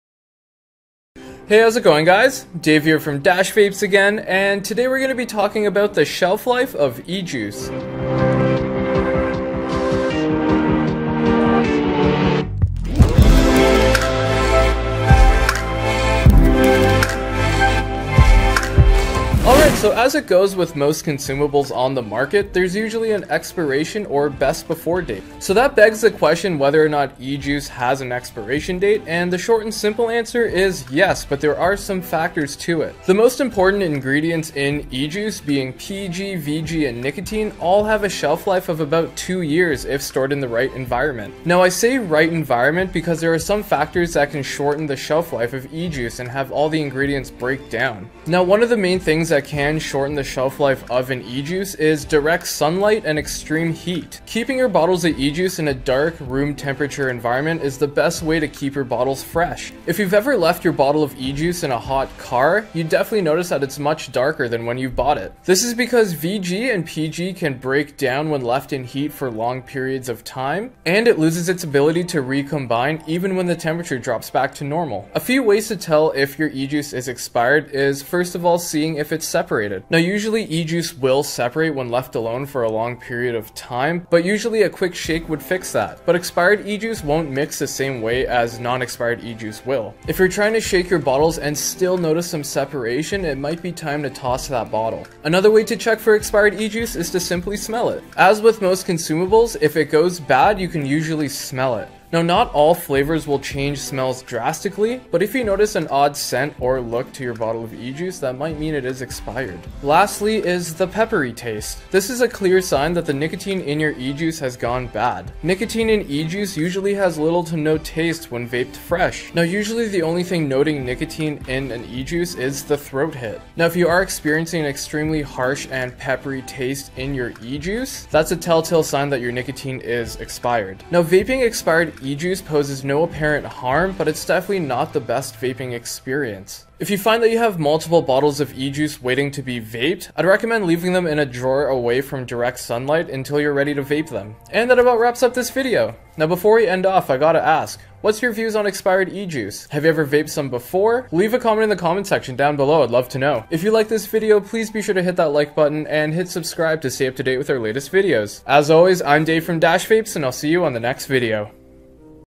هل لسوائل التبخير فترة صلاحية؟
هل تعتبر سوائل التبخير سامة بعد انتهاء فترة صلاحيتها المفترضة؟
كل الشكر لقناة Dash Vapes